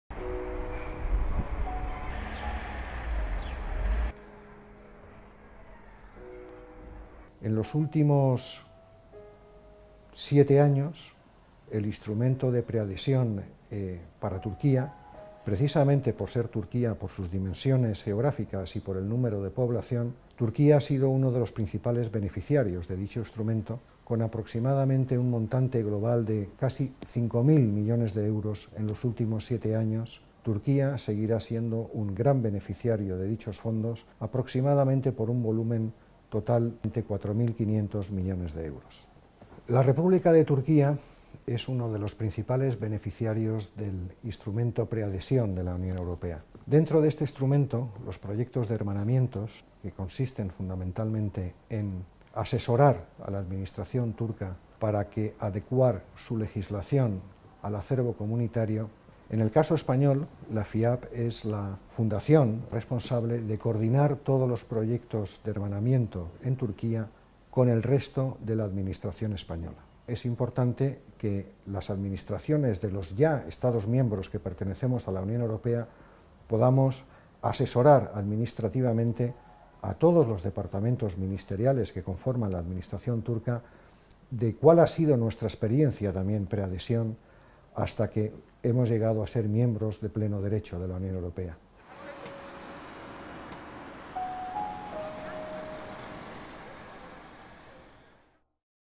Entrevistas FIIAPP_Cristobal González-Aller
El ex Embajador de España en Turquía nos habla sobre la Pre-Adhesión de Turquía a la Unión Europea y la colaboración de la FIIAPP en este trabajo.